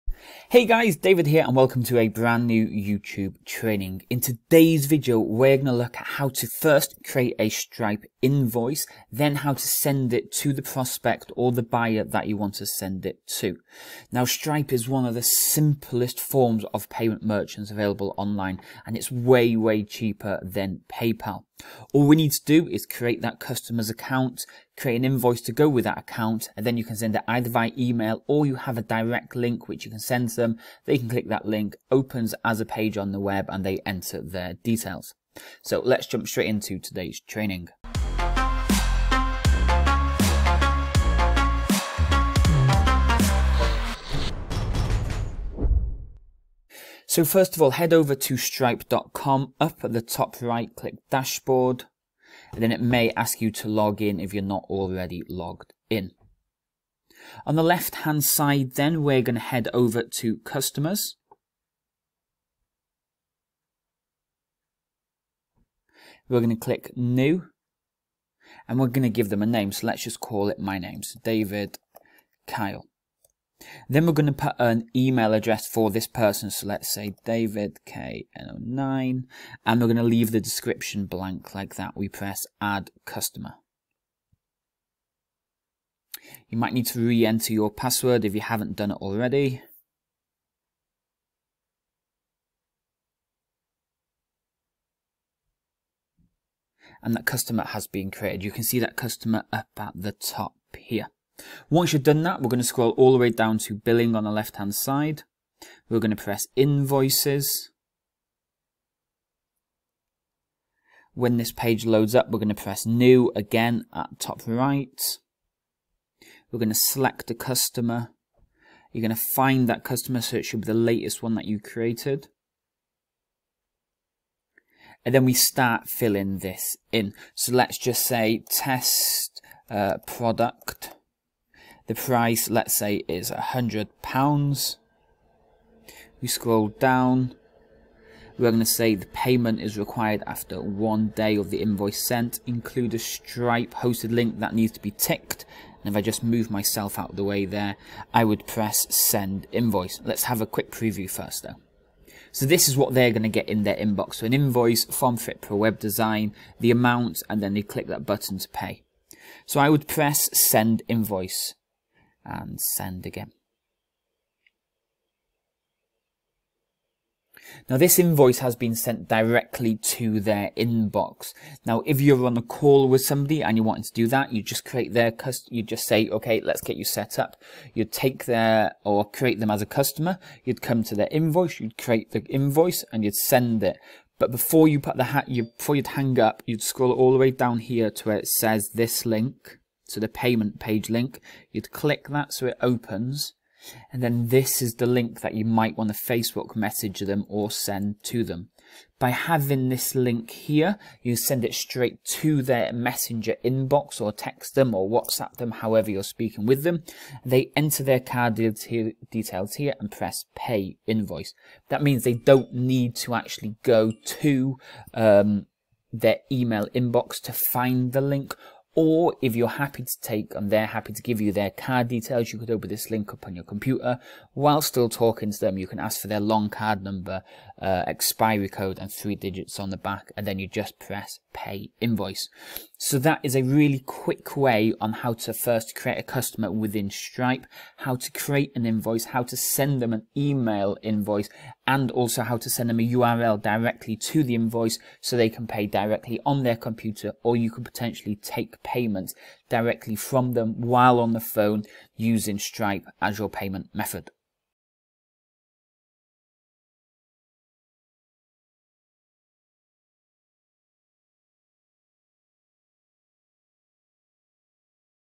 How To Create An Invoice In Stripe | Step by step to creating a customer invoice in Stripe
Welcome to another quick YouTube training with me David Kyle :)

In this super quick video, I am going to show you how to create first a new customer in Stripe, then how to create the invoice in Stripe, and finally how to send the invoice via email and direct URL to your customer.

Sending out invoices is key to any business, this is why they need to be easy to create and send. Plus giving them a simple option to pay online should speed the payment up.

Follow these easy step in the videos to create your first invoice in Stripe.

- The FREE FitPro Lead generation with Dave! Group